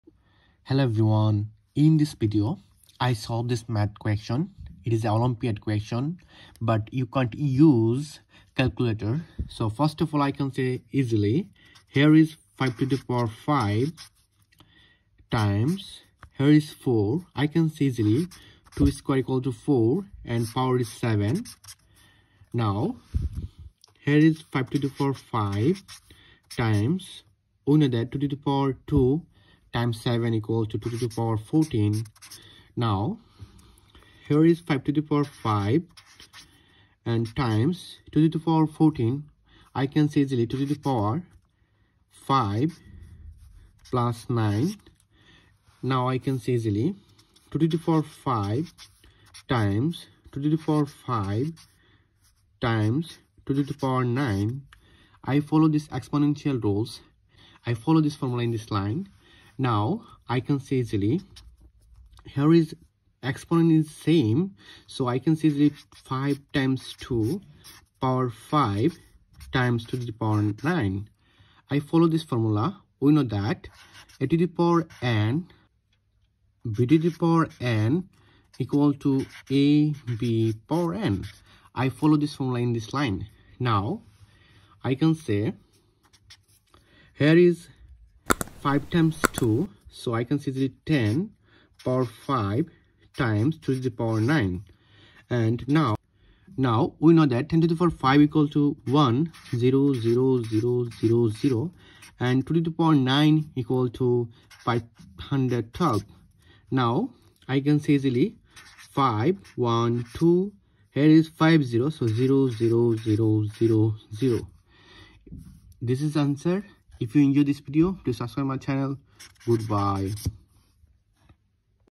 Olympiad Mathematics | You Should Be Able To Solve This Olympiad Math 5⁵ × 4⁷
Hello everyone ,In This video I solve a math olympiad question ,this question is 5⁵ × 4⁷
But you solve it without calculator. I solve it without calculator. Have a good day my all family.